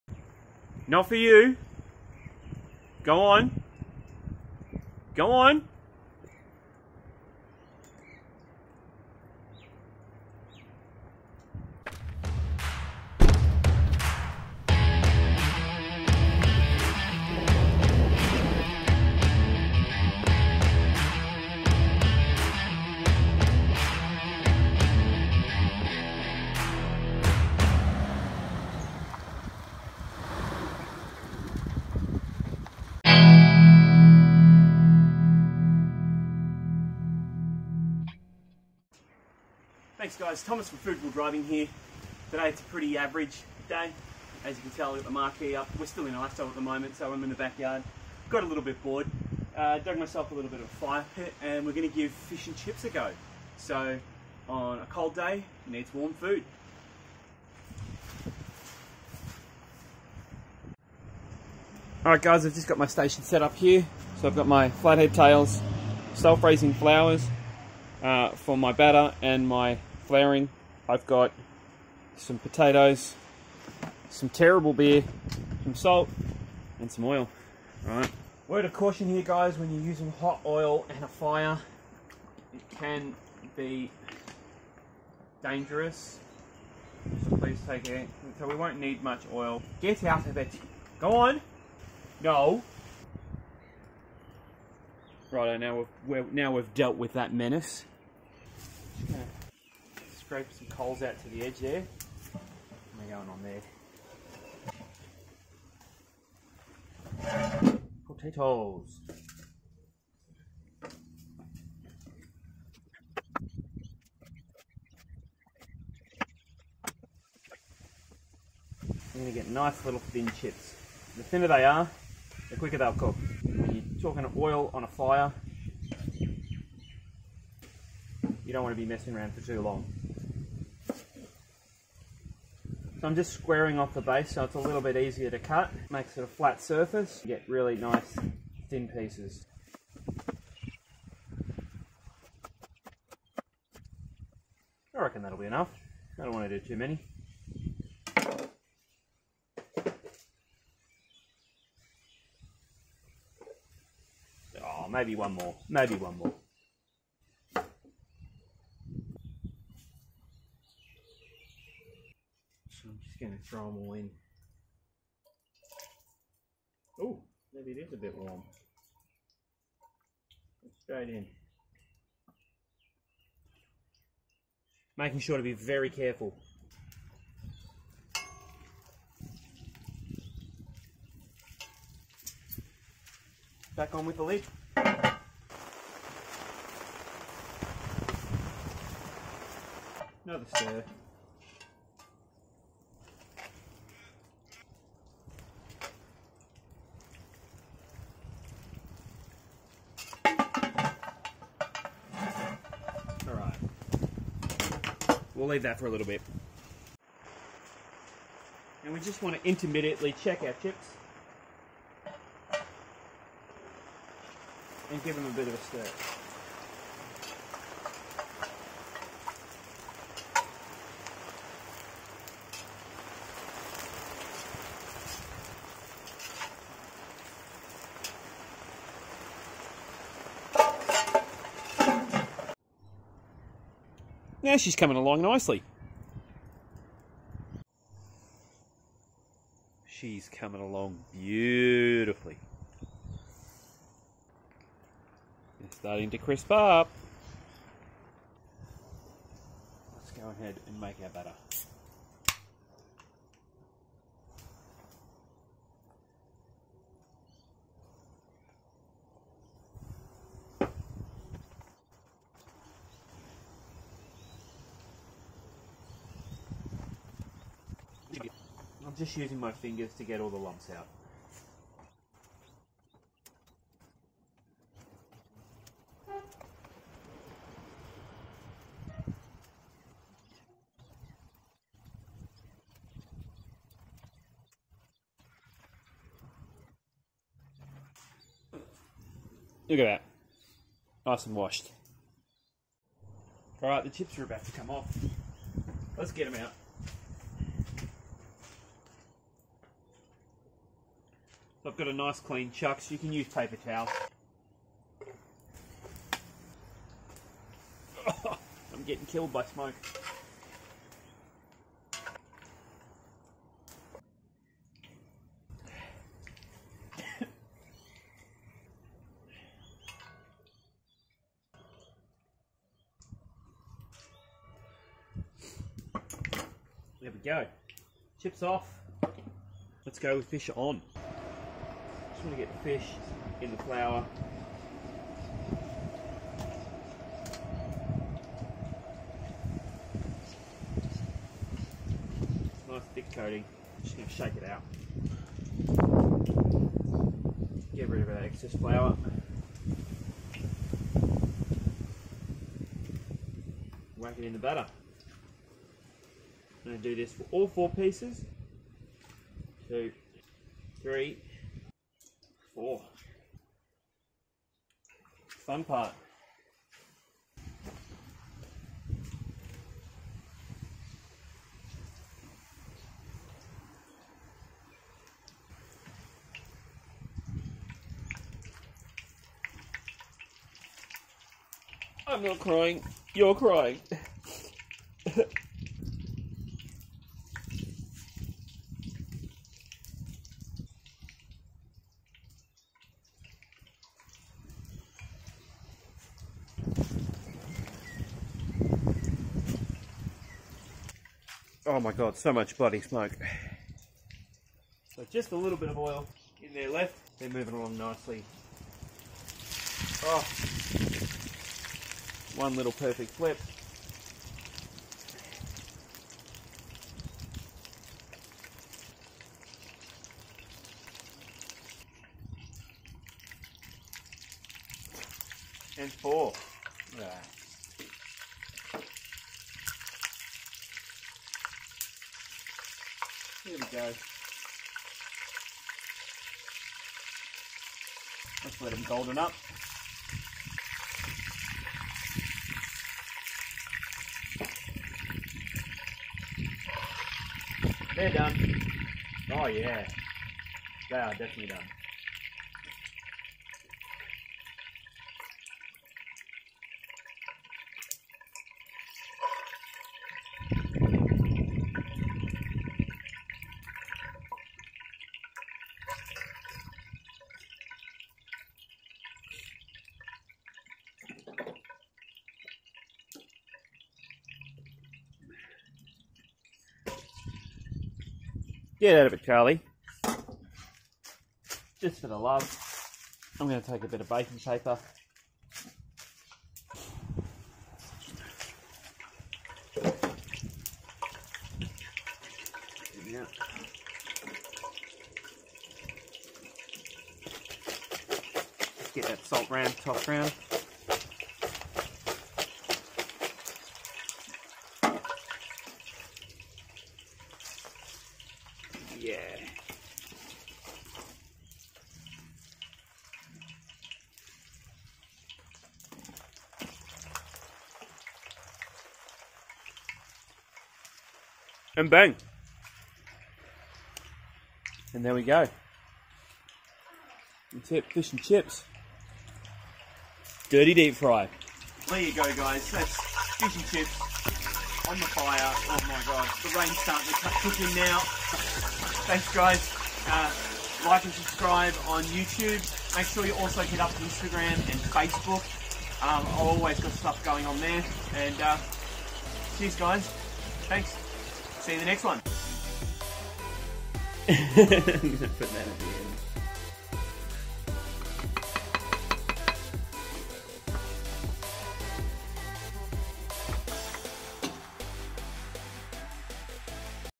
Iso Fish & Chips HD
Once again still stuck at home, sorry for the train noise.
Here is my fish and chips done on the fire with a camp oven.

Category
People & Blogs